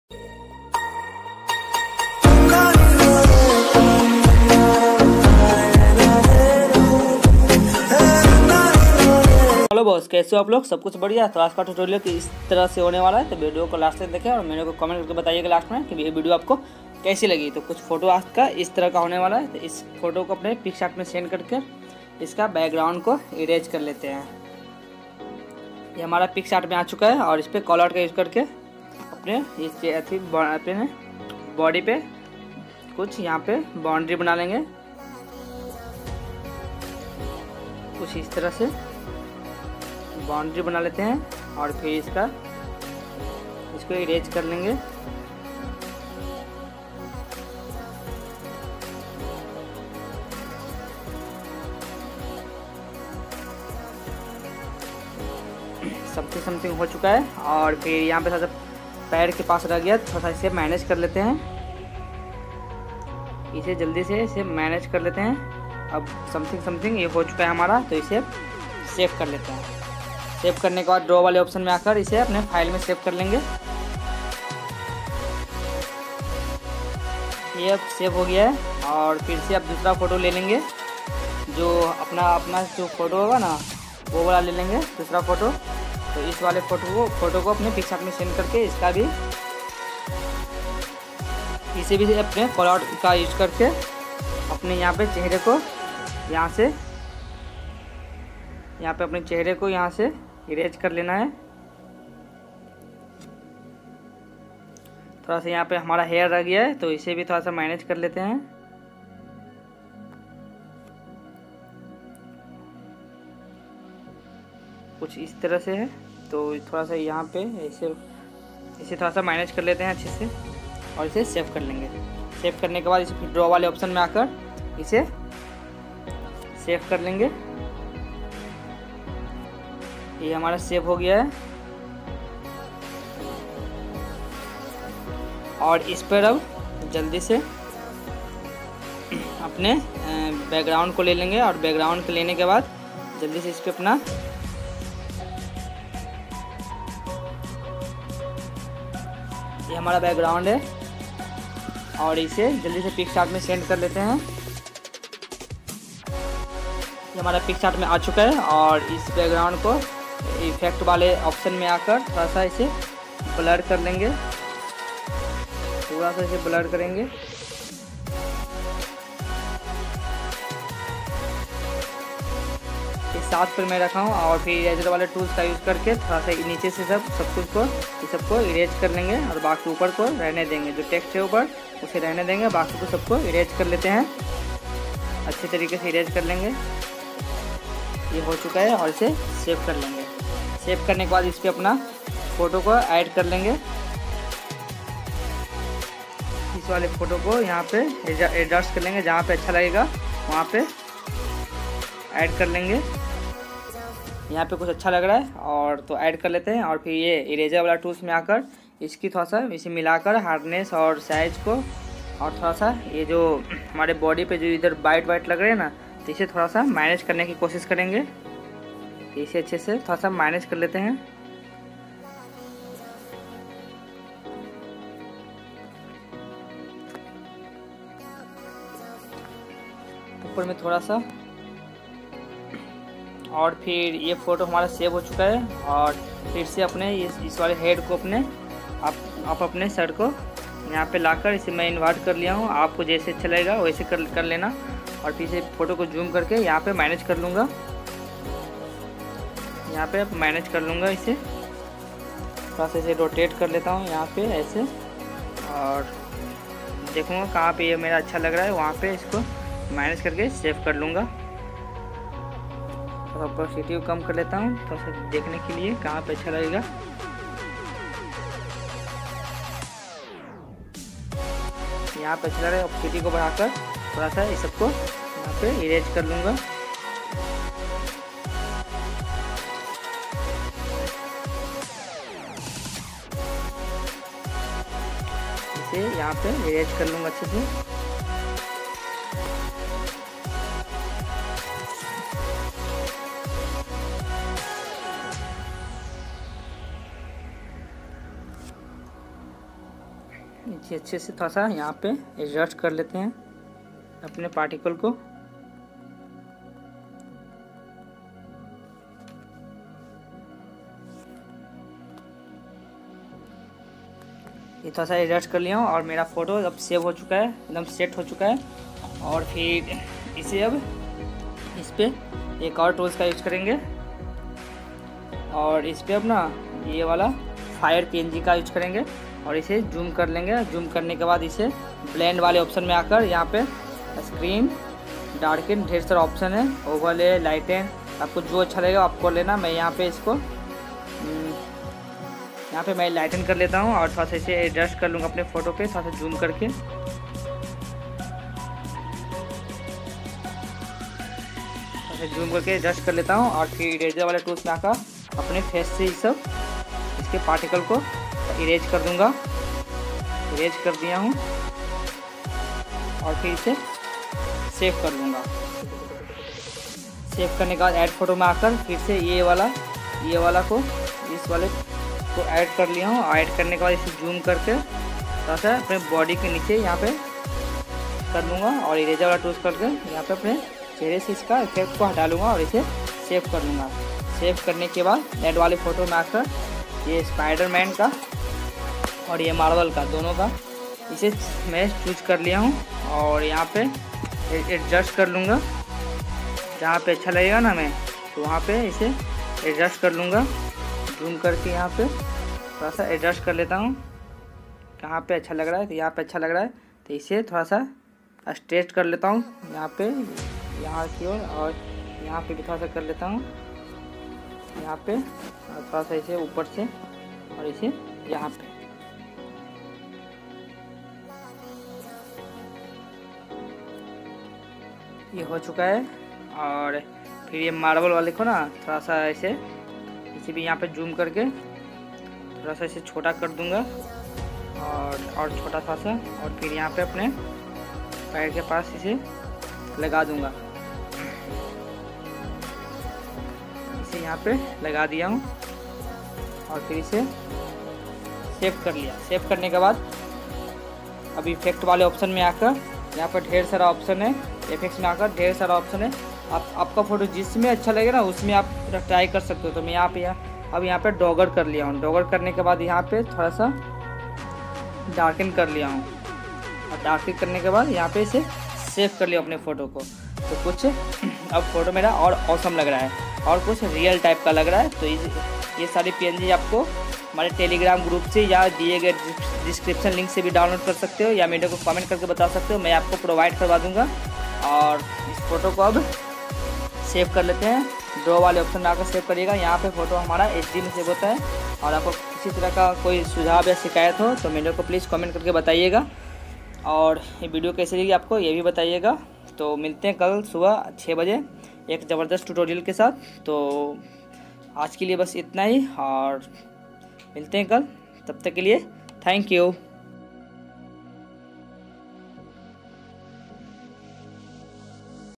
Spider Man Photo Editing Viral Concept | Picsart Spider Man Tutorial
Spider Man Photo Editing Viral Concept | Picsart Spider Man Photo Editing Tutorial |- S5 Arches
Rahul Sharma:
[ PicsArt, Snapseed, Lightroom, Autodesk & Many More ]

My Instagram I'd👇
rahulsharma2163.

photoshotboy ThankYou 🙏